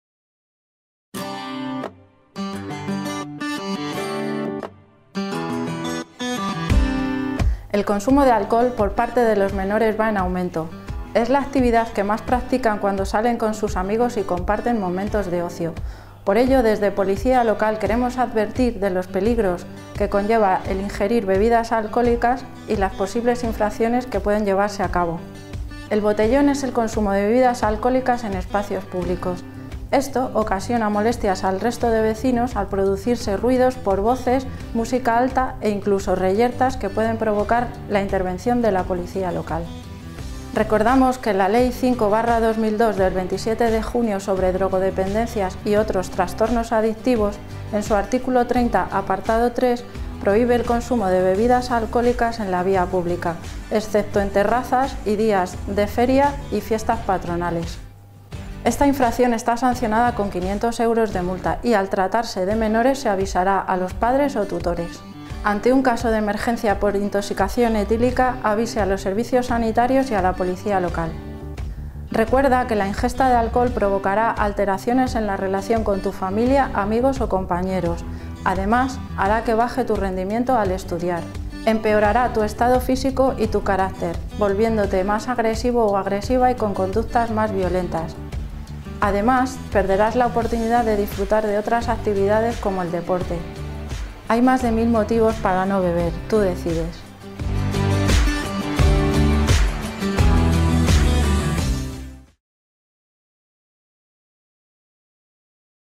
Recomendaciones de Policía Local de Leganés: Alcohol y menores
La Policía Local de Leganés informa a los ciudadanos sobre diversos aspectos de las ordenanzas y campañas municipales, con el objetivo de concienciar sobre las mismas y conseguir una mejor convivencia.

En este vídeo se informa sobre el reglamento municipal que regula el consumo de bebidas alcohólicas en lugares públicos, haciendo especial mención a la circunstancia de cuando los consumidores son menores de edad.